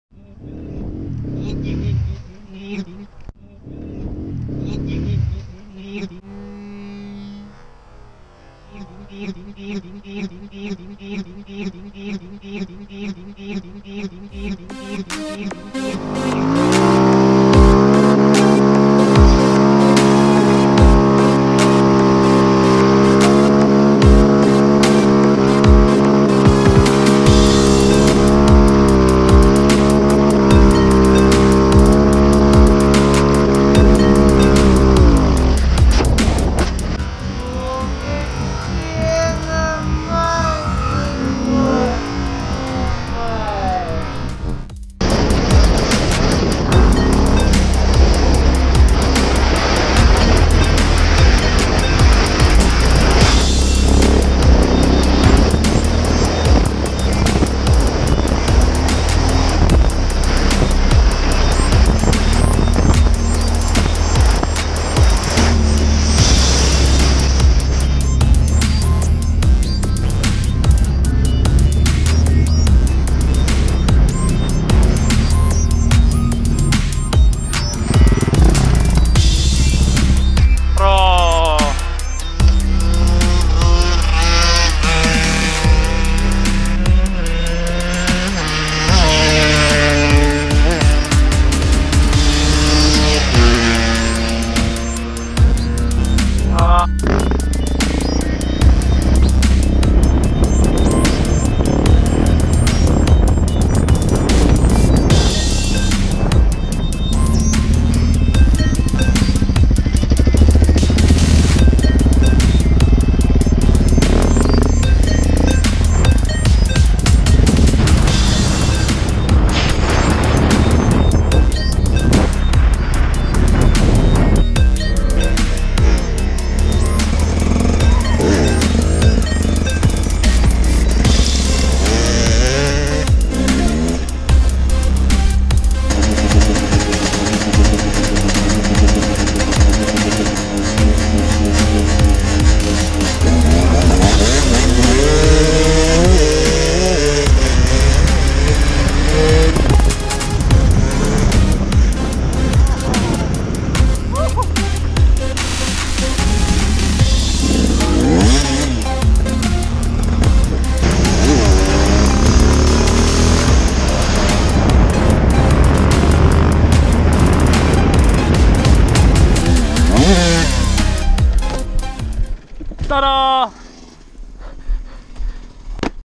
Kesän jämät
Järkevämpääkin videota oisi voinu tehdä mutta kovalevy leipo ja sen mukana meni suurin osa kesän kuvauksista..